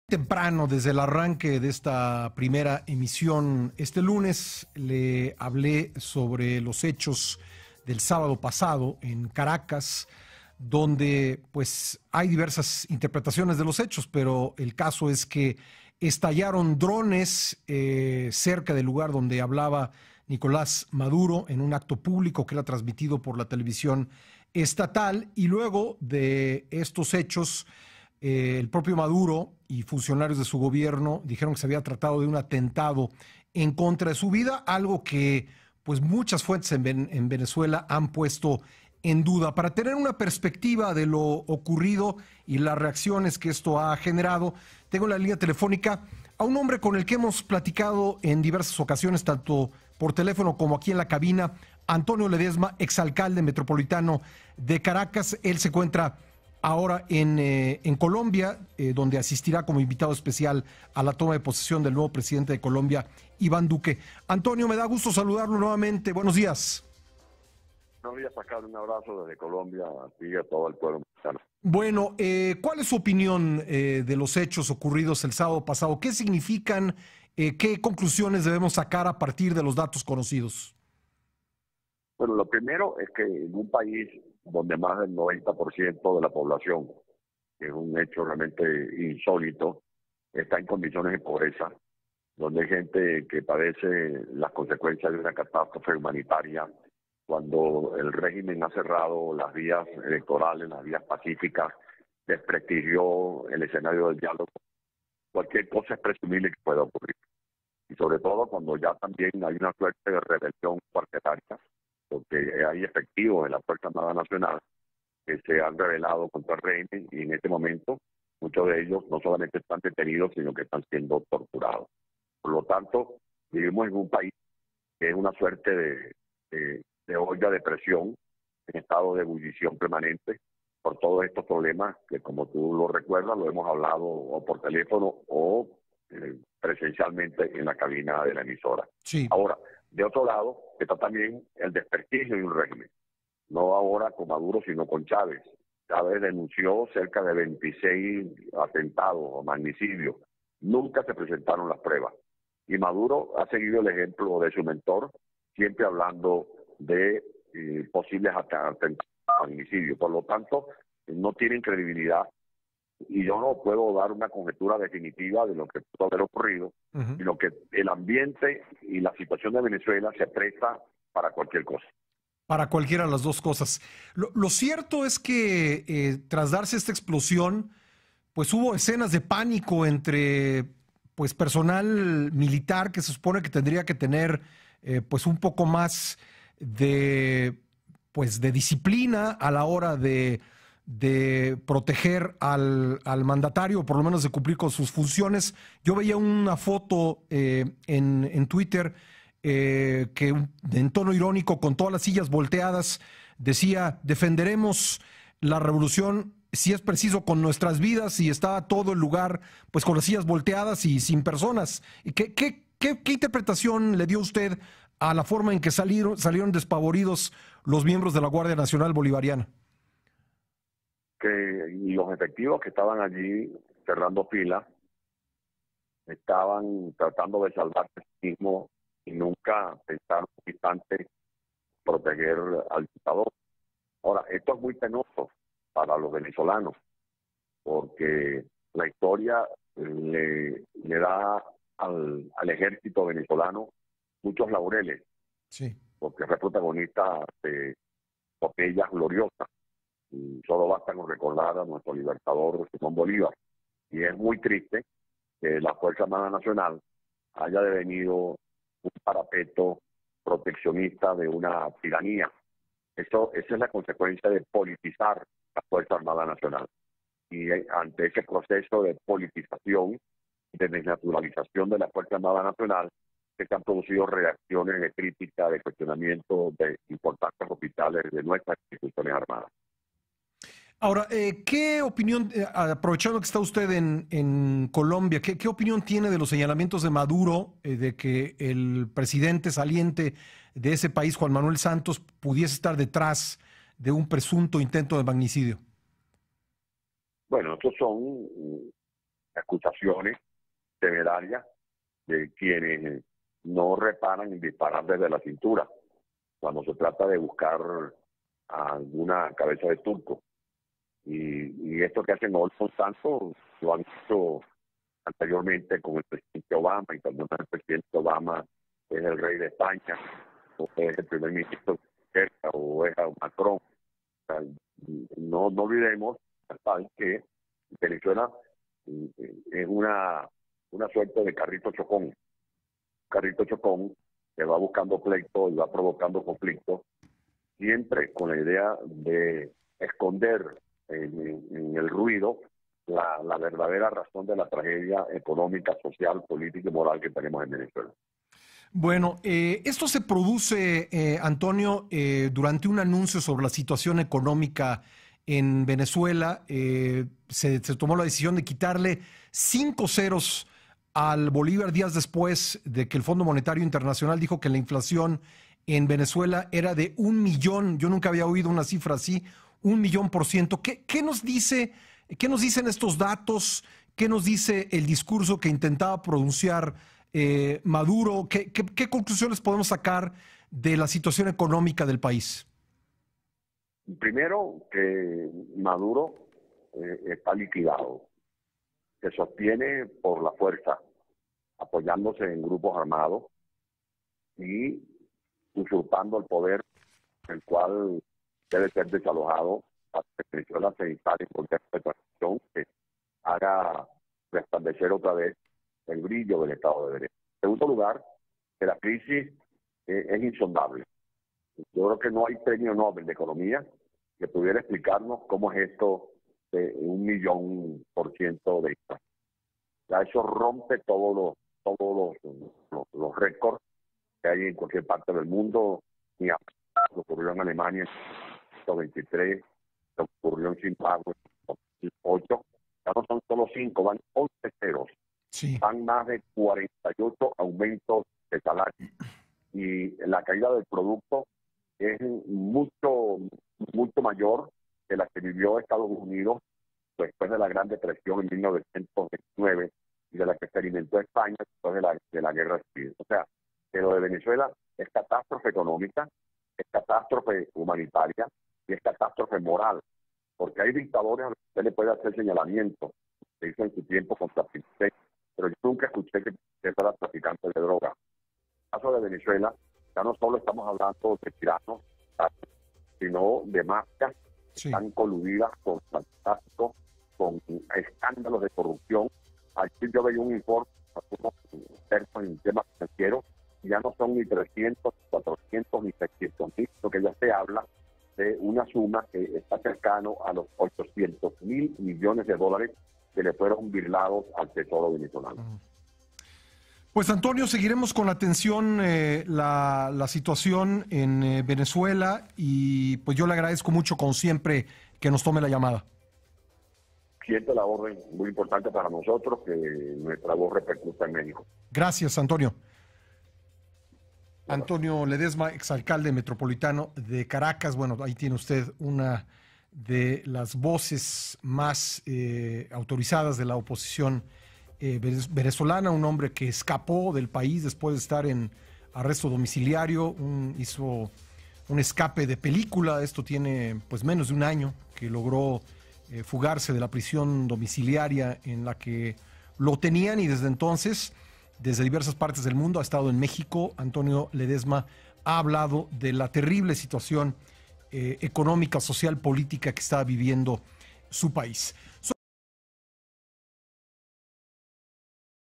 No hay credibilidad en versión de Maduro sobre supuesto atentado: Antonio Ledezma / Primera Emisión
Antonio Ledezma, exalcalde Metropolitano de Caracas, habla en entrevista sobre las versiones que se han presentado luego del supuesto atentado contra el presidente de Venezuela, Nicolás Maduro.